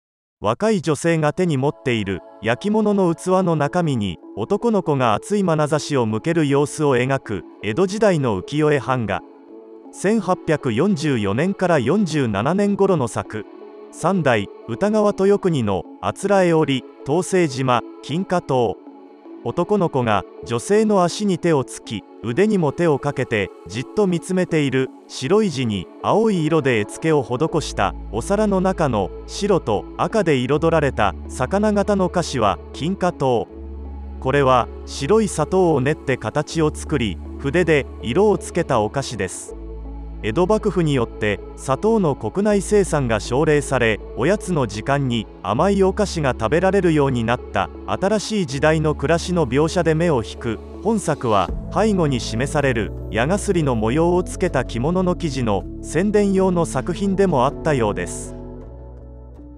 おいしい美術83🐟三代 歌川豊国《誂織当世島 金華糖》1844-47年
前回の動画はコチラ➡おいしい美術82🐶歌川広景《江戸名所道化尽 九 湯嶋天神の台》1859年

「おいしそうな飲食物」を表した 美術 作品をとりあげる本シリーズ、第83回は三代 歌川豊国《誂織当世島 金華糖》（1844-47年頃）。少年が見つめる、江戸時代の色あざやかなお菓子を表し、縞柄の着物の生地の宣伝もおこなった 浮世絵 版画について、細部を拡大しながら詳しく解説します。

その他の「おいしい美術」動画はこちらの再生リストからどうぞ。